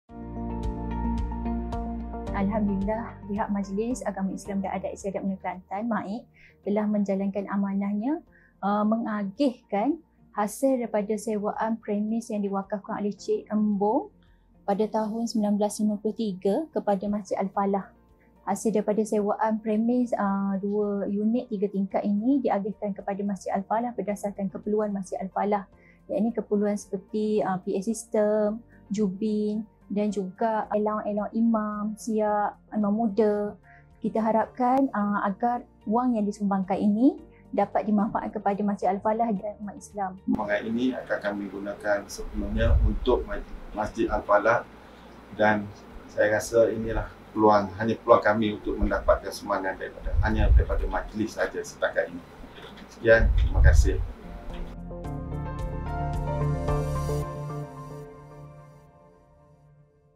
𝐌𝐀𝐍𝐅𝐀𝐀𝐓 𝐖𝐀𝐊𝐀𝐅 𝐔𝐍𝐓𝐔𝐊 𝐌𝐀𝐒𝐉𝐈𝐃 𝐀𝐋-𝐅𝐀𝐋𝐀𝐇 𝐖𝐀𝐊𝐀𝐅 𝐒𝐈𝐊𝐔 𝐊𝐎𝐓𝐀 𝐁𝐇𝐀𝐑𝐔
Masjid Al-Falah Wakaf Siku, Kota Bharu telah menerima sumbangan wakaf Majlis Agama Islam dan Adat Istiadat Melayu Kelantan (MAIK) bernilai RM32,360 hasil daripada hartanah yang diwakafkan oleh Hajah Embong binti Che Mamat.
Penyerahan replika cek telah disempurnakan oleh YBrs. Puan Shakirah binti Mohamad, Pegawai Wakaf MAIK kepada YBrs. Dr Fathi bin Haji Abdullah, Pengerusi Masjid Al-Falah Wakaf Siku, Kota Bharu yang diadakan di MAIK.
Hadir sama, YBrs. Encik Ahmad Rozman bin Ismail, Setiausaha Masjid Al-Falah Wakaf Siku, Kota Bharu dan YBrs. Puan Siti Nurhidayu Binti Othman, Penolong Pegawai Wakaf, MAIK.
Jom sama-sama kita merebut peluang berwakaf bersama-sama dengan MAIK dengan klik pada pautan disediakan di bawah: